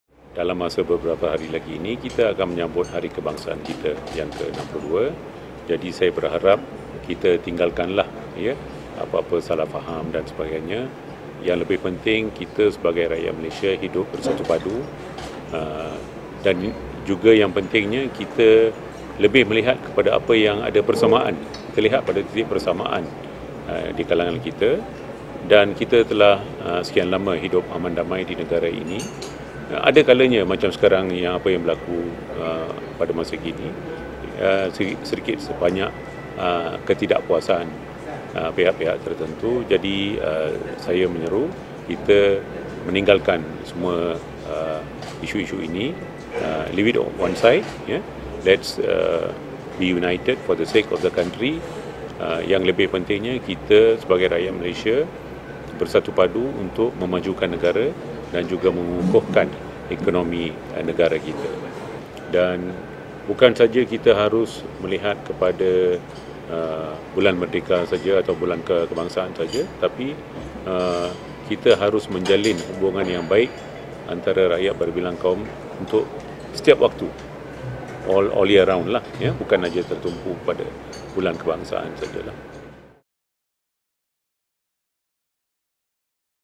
Waytha Moorthy: Set aside differences and stay united
National Unity and Social Wellbeing Minister P. Waytha Moorthy expressed his hope on Saturday for Malaysians to put aside their differences and focus on the upcoming National Day celebration.